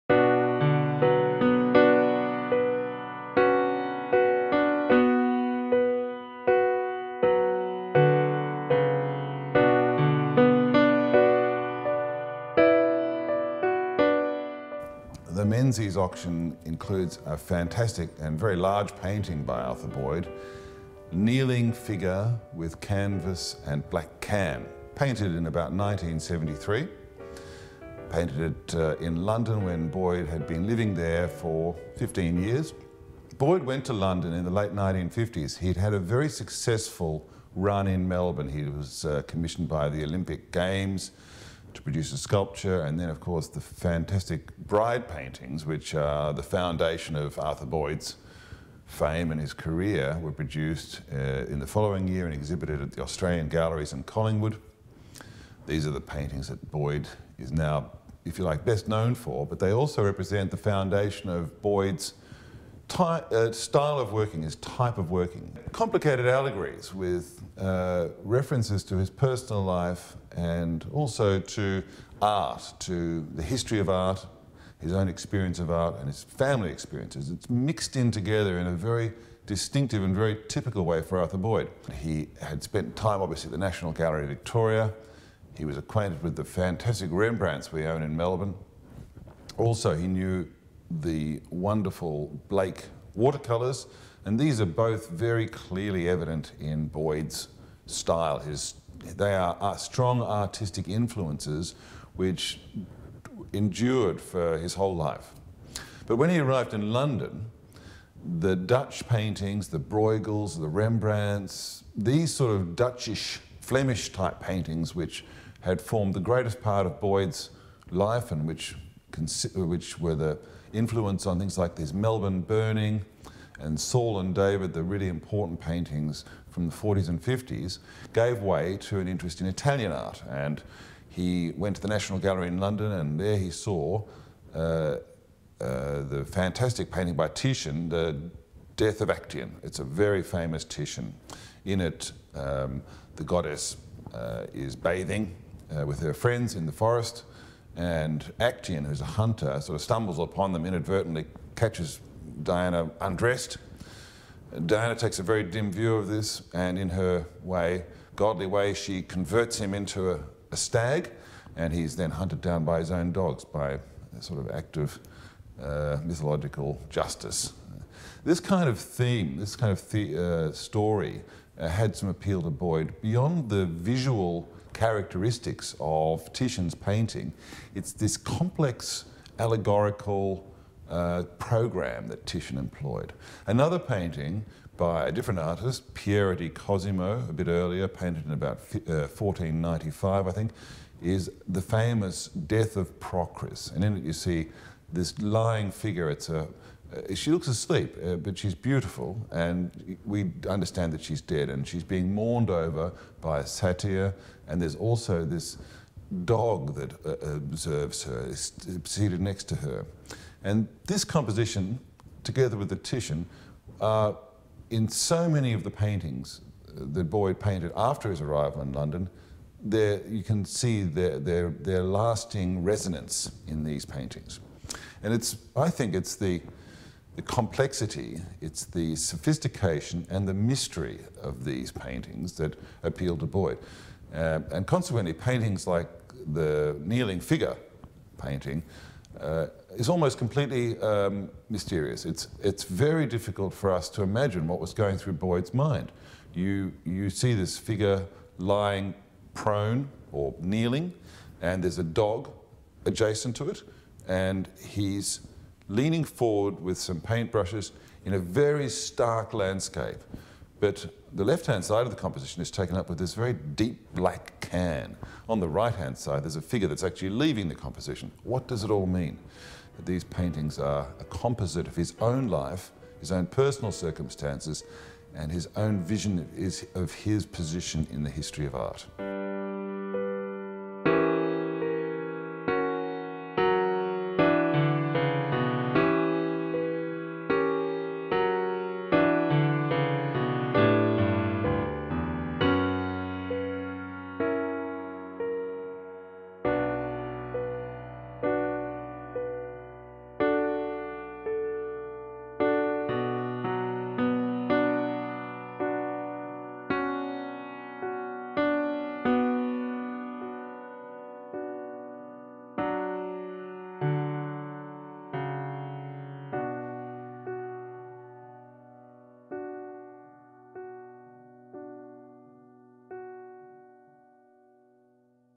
Menzies September 2016 Auction Arthur Boyd
Arthur Boyd's 'Large Kneeling Figure with Canvas and Black Can' 1973 is a work with many possible interpretations. Menzies' Head of Art Tim Abdallah talks about some of them in this video introducing the piece which will be offered for auction for the first time on September 21.